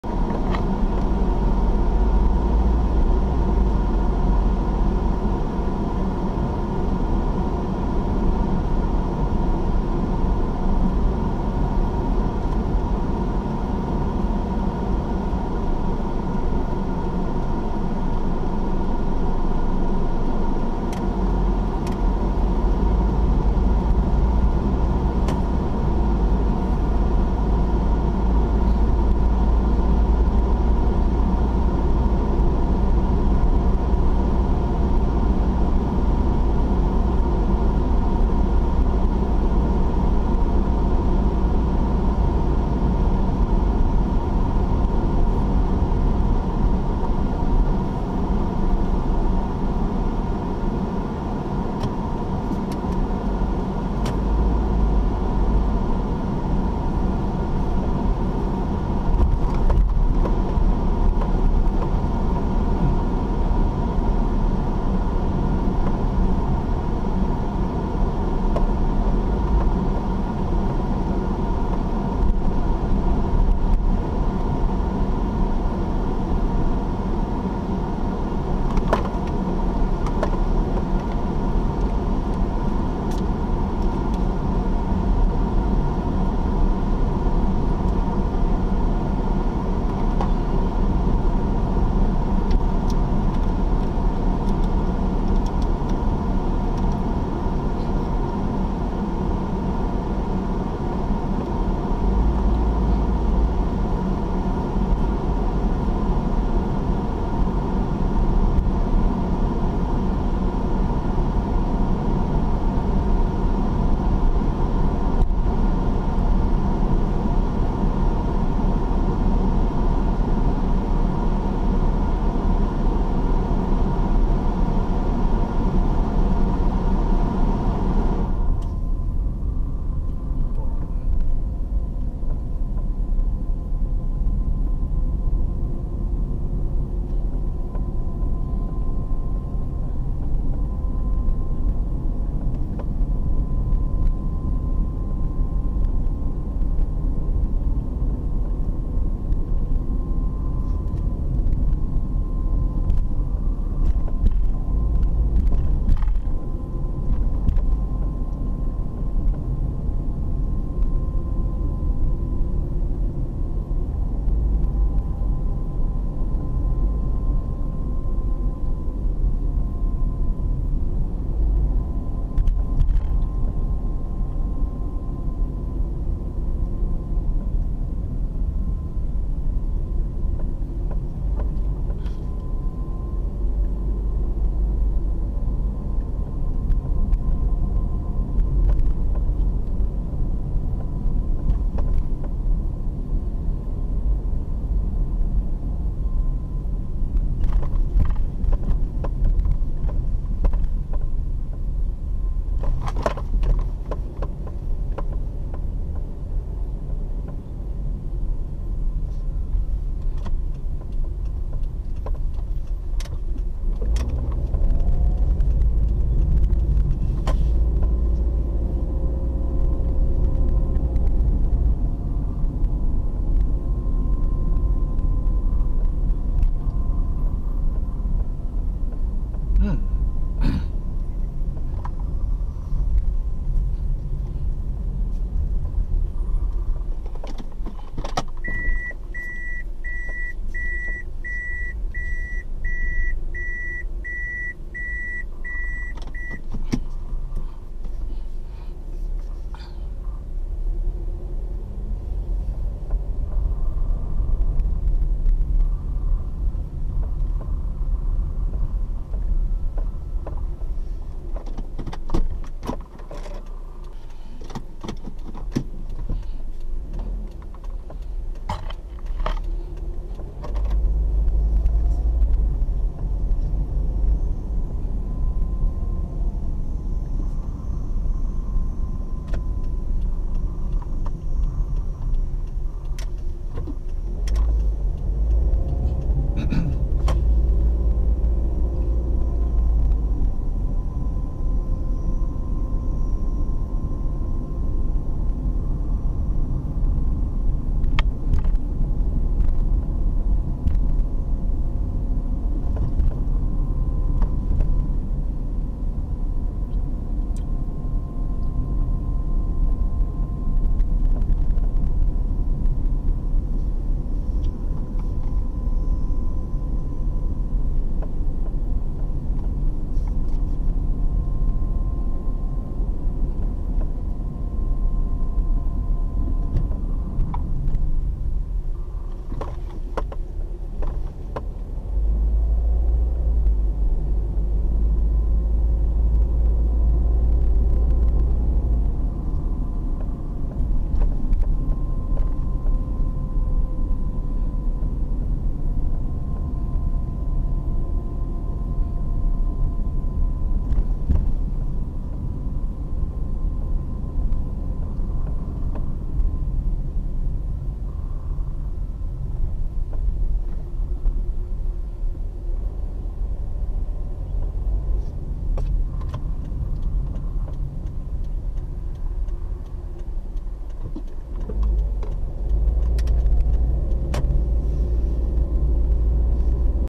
[Suburbs] Cathedral Hill Suburb, United Nations Avenue | November 2020
*** NOTE: Sorry for the noise coming from the air conditioner---it has been insanely hot these last few days. On this particular day, the highest temperature recorded by the vehicle was 39 degrees celsius.

Exploring Longacres/Cathedral Hill, in Lusaka, Zambia. This footage was taken on Saturday November 7 2020.

## Timeline
00:00:00 United Nations Avenue

## Footage Metadata
* Time: 12H20--14H50
* Date: November 7 2020